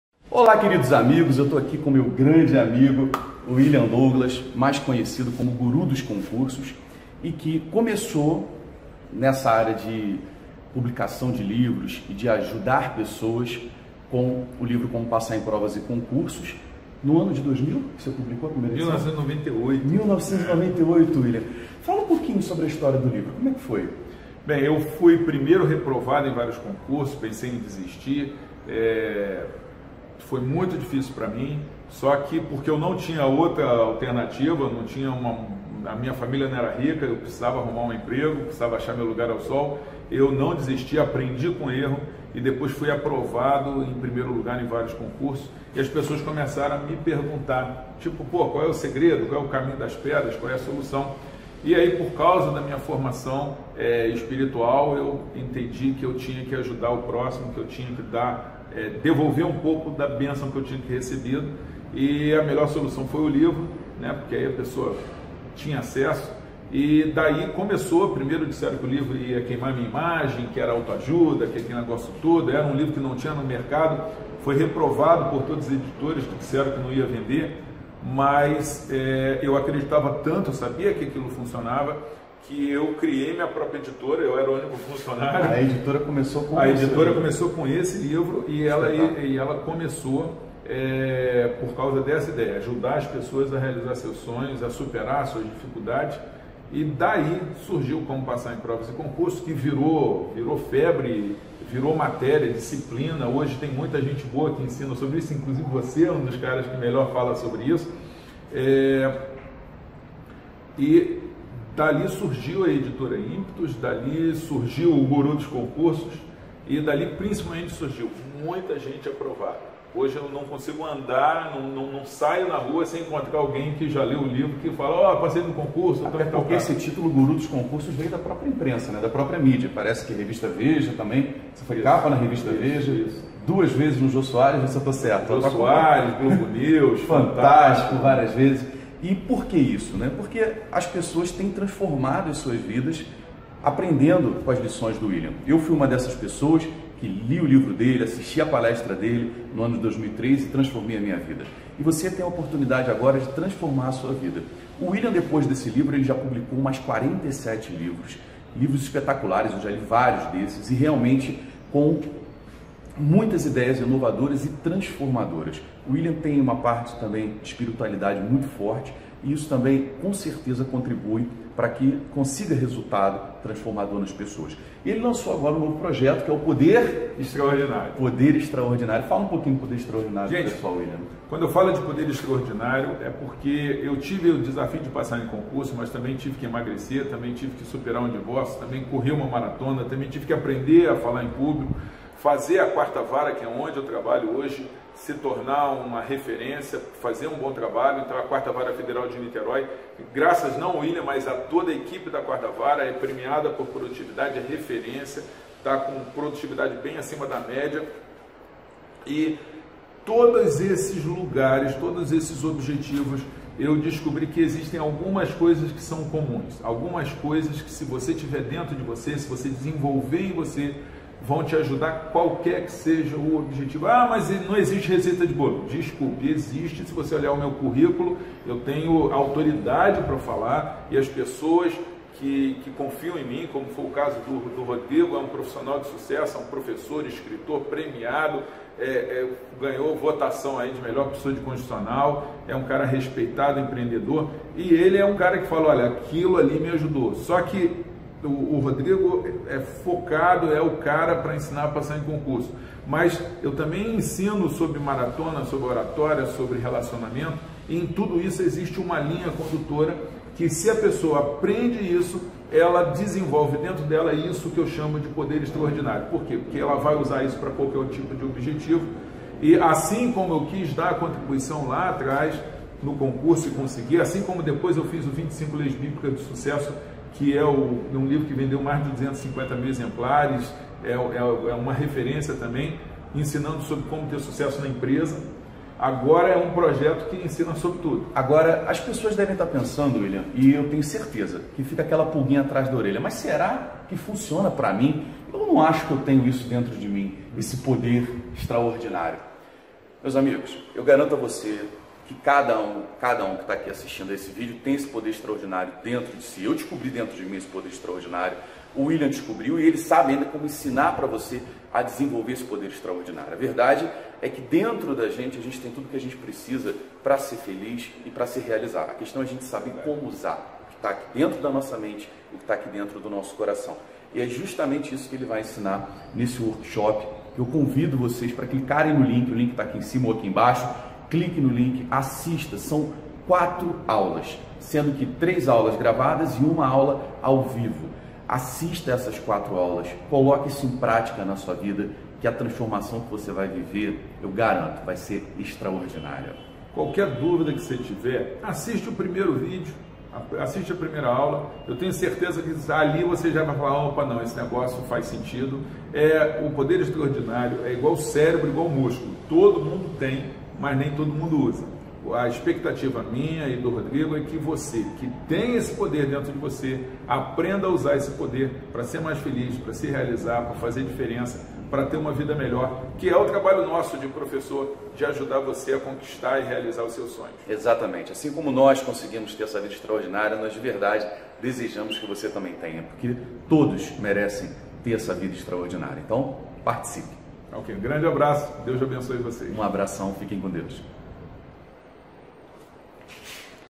Conheça a história do livro "Como Passar em Provas e Concursos" do William Douglas
William Douglas conta sobre como surgiu o livro que tem ajudado milhares de concurseiros a mudarem suas vidas. Participe do Workshop gratuito PODER EXTRAORDINÁRIO com William Douglas

Eu li o livro em 2003 e foi fundamental para eu passar meu passar no concurso da PRF em 2003.